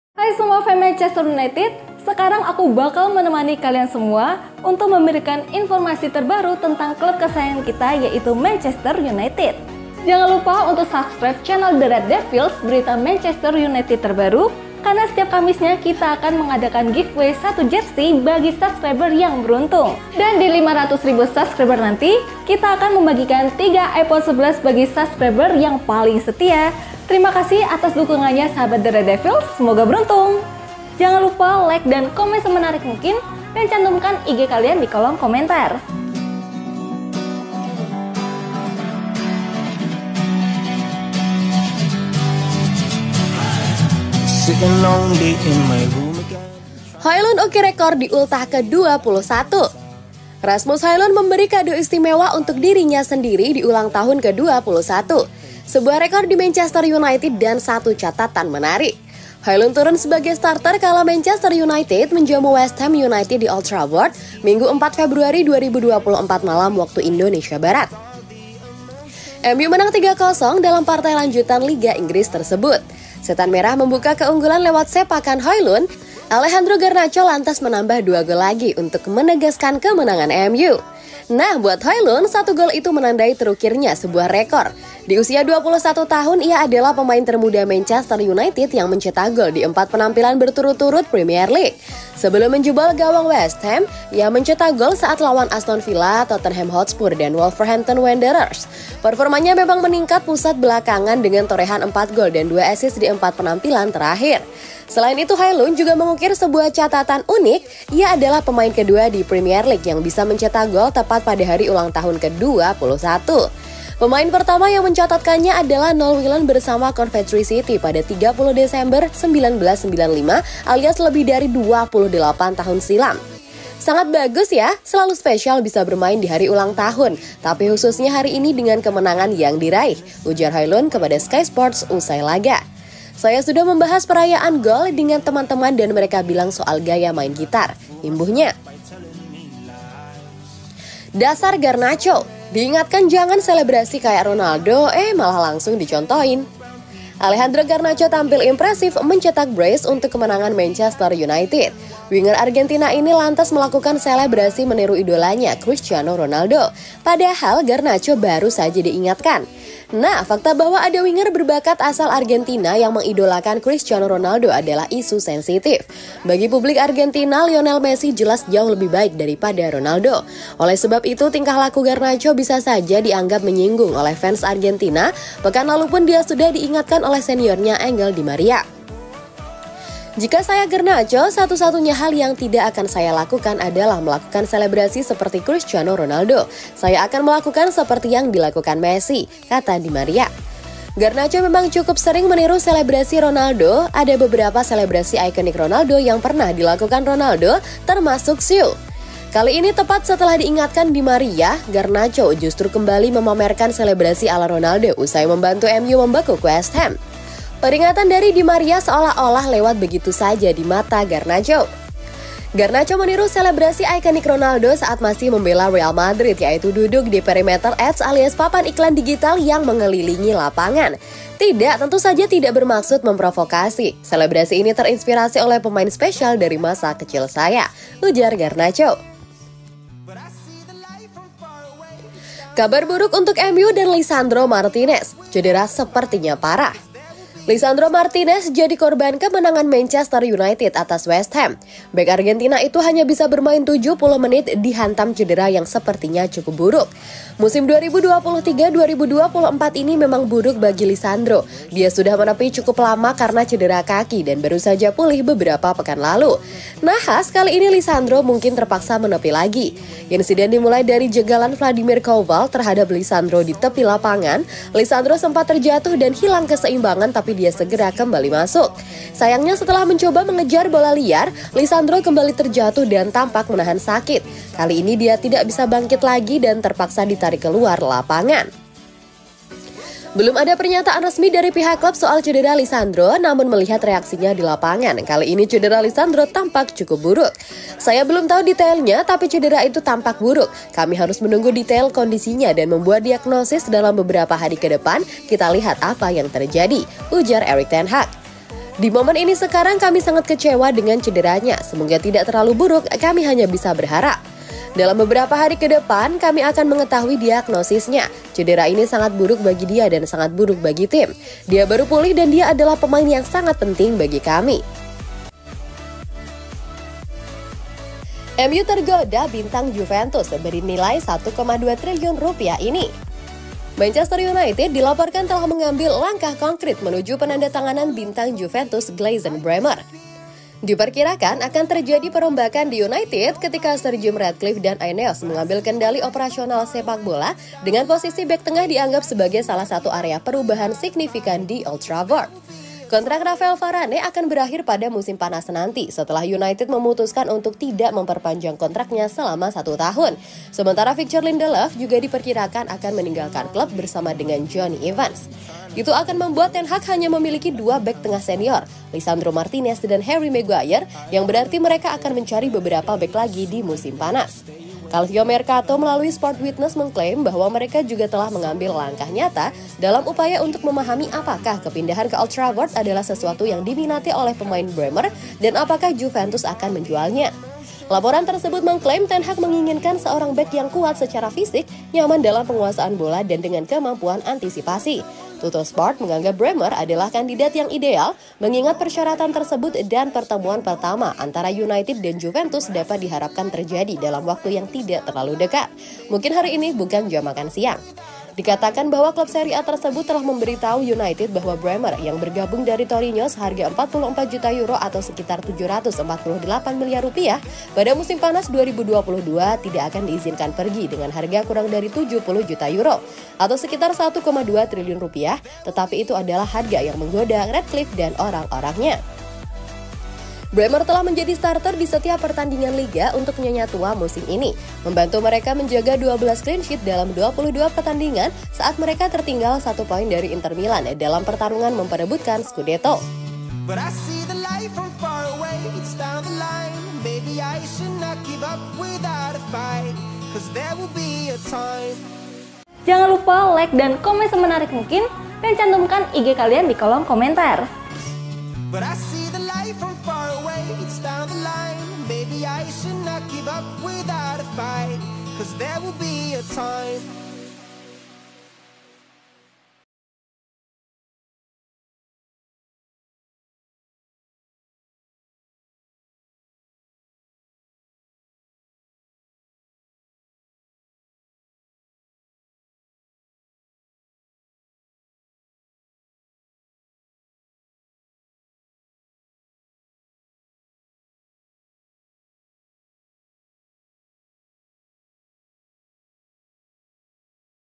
MANTAP ✅ RASMUS HOJLUND CETAK REKOR BARU 🤩 GARNACHO TIRU CR7 LAGI 🔴 Berita MU
BERITA SEPAK BOLA TERKINI & TERBARU HARI INI SERTA HASIL PERTANDINGAN BOLA TADI MALAM DARI The Red Devils - Berita Manchester United Terbaru Hari Senin, 05 Februari 2024  :